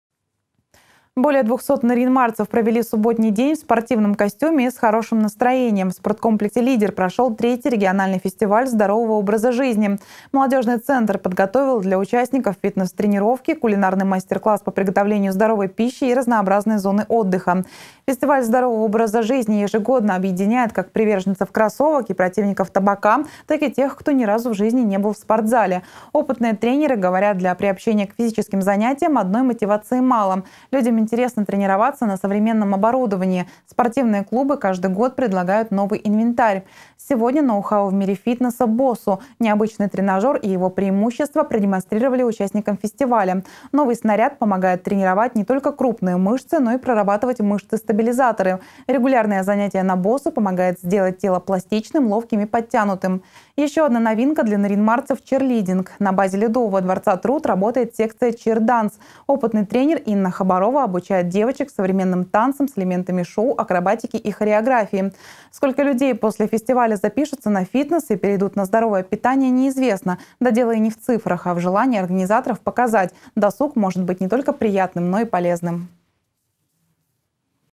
В Нарьян-Маре прошел фестиваль здорового образа жизни
Более 200 нарьянмарцев провели субботний день в спортивном костюме и с хорошим настроением. В спорткомплексе «Лидер» прошел 3-й региональный фестиваль здорового образа жизни. Молодежный центр подготовил для участников фитнес-тренировки, кулинарный мастер-класс по приготовлению здоровой пищи и разнообразные зоны отдыха.